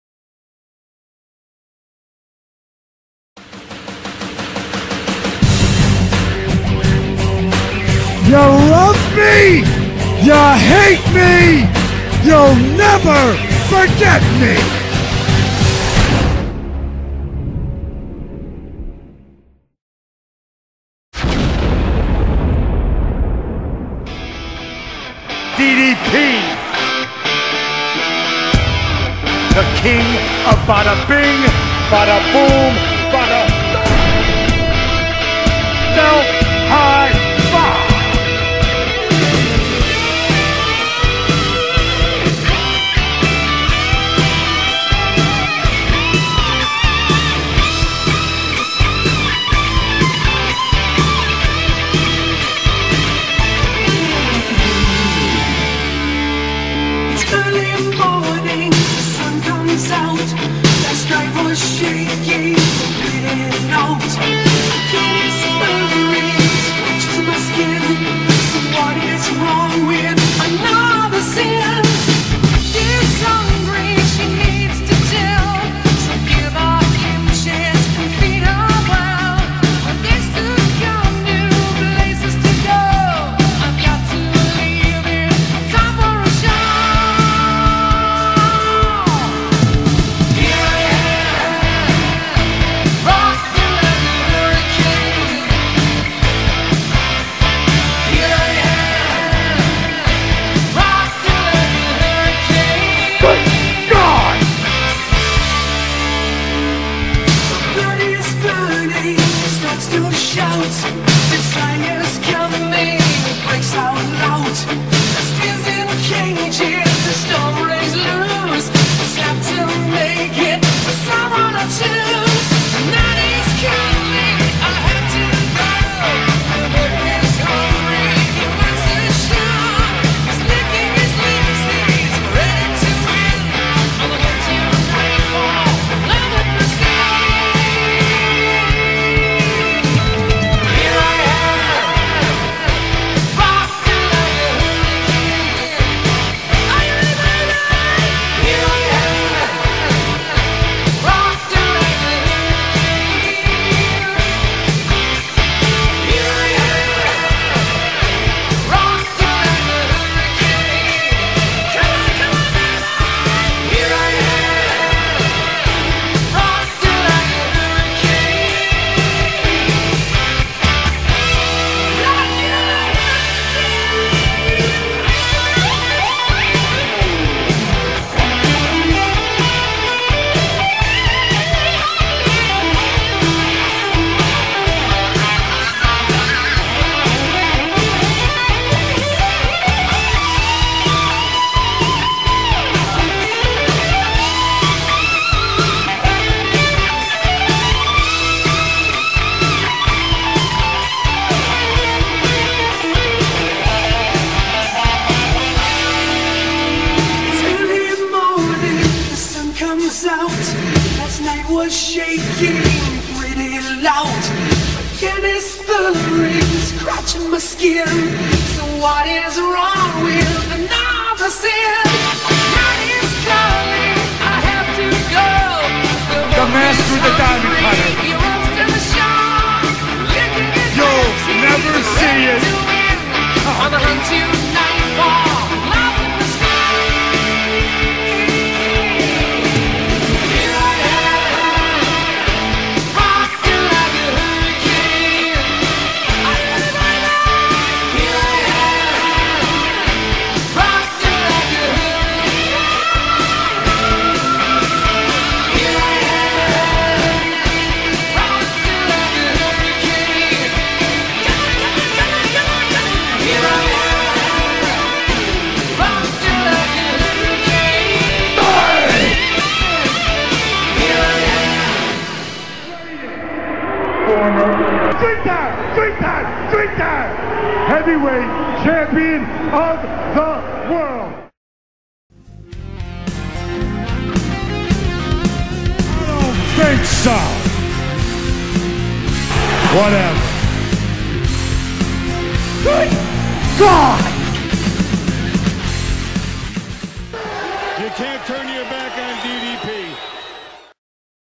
Feel The Diamond Cutter? - DDP Tribute
Diamond Dallas Page. A tribute video for the former 3 time world champion, the master of the Diamond Cutter, the King of Bada-Bing, the original People's Champion... DDP. He's my all-time fav wrestIer. Many say he is one of the greatest versatile wrestIers of all-time, you agree? (I know you do) ^_^

I've edited a very similar video to this one last year. The original one was silenced by YTube, because I used the actual version of the song. I used a cover version this time. Inspired by the 'TNT Diamond Cutter Reel' and the 'Diamond Cutters on Everyone' videos.